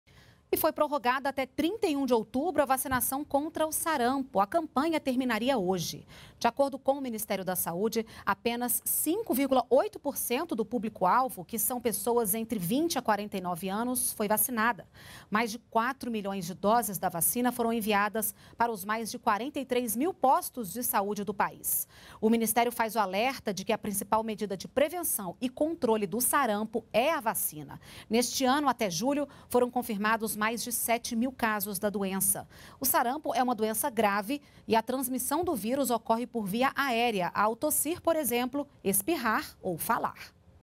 Vacinação contra o sarampo é prorrogada até 31 de outubro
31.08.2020 -./ A campanha terminaria hoje./ (ENTRA IMAGEM) De acordo com o Ministério da Saúde, apenas 5 vírgula 8 por cento do público-alvo, que são pessoas entre 20 a 49 anos, foi vacinado./